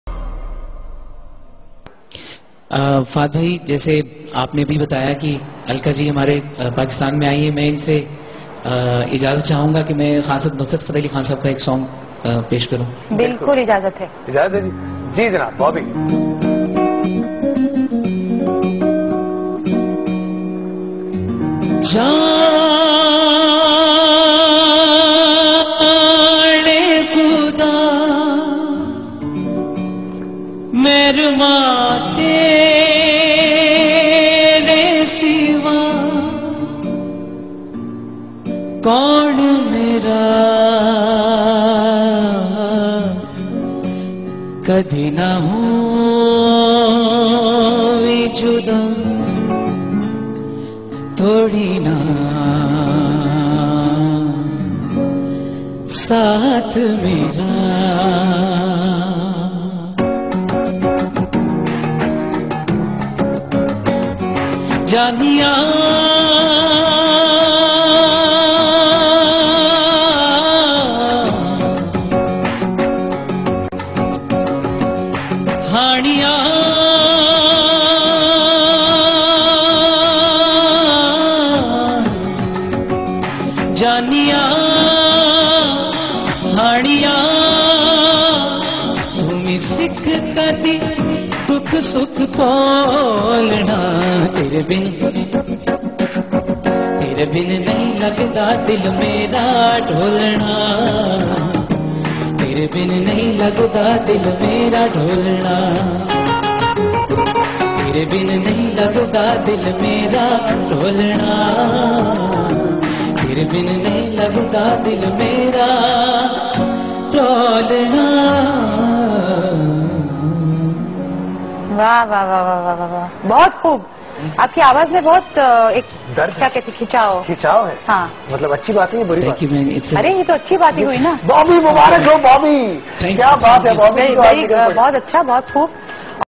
Performance by Furqan Ahmad Bobi with Alka Yagnik and Fahad Mustafa@HUMTV
Performance by Furqan Ahmad Bobi with Alka Yagnik and Fahad Mustafa @HUM TV Amazing talent shown by a Pakistani visually impaired/Blind Musician,singer,music composer,voiceover artist,mimicry artist. Furqan Ahmad Bobi in the presence of alka yagnik with fahad mustafa @HUMTV ￼humtv